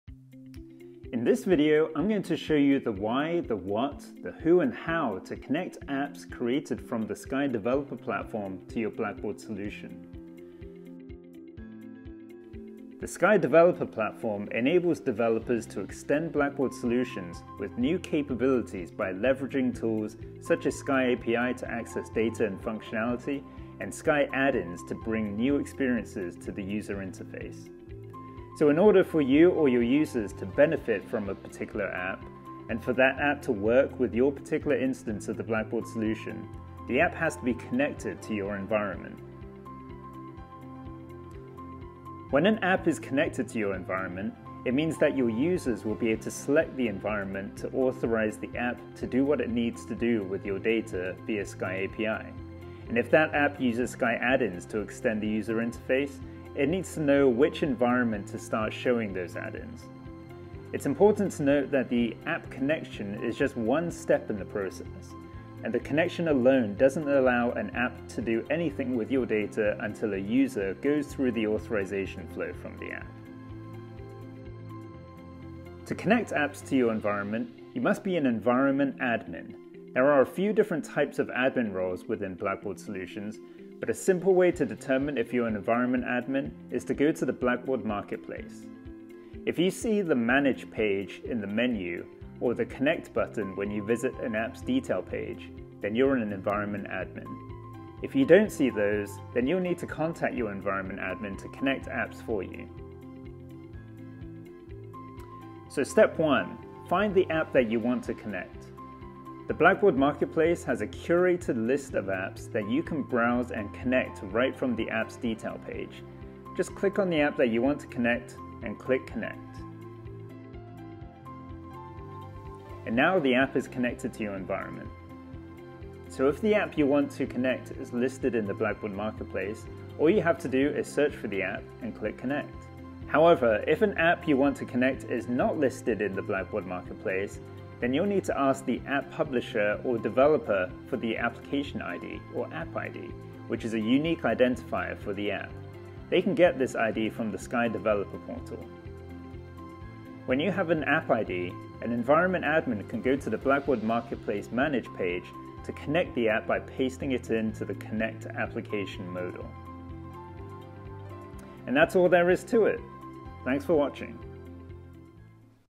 How to connect applications to your Blackbaud environment
In this video, we show you the why, what, who and how to connect apps created from the SKY Developer platform to your Blackbaud solution to amplify your impact.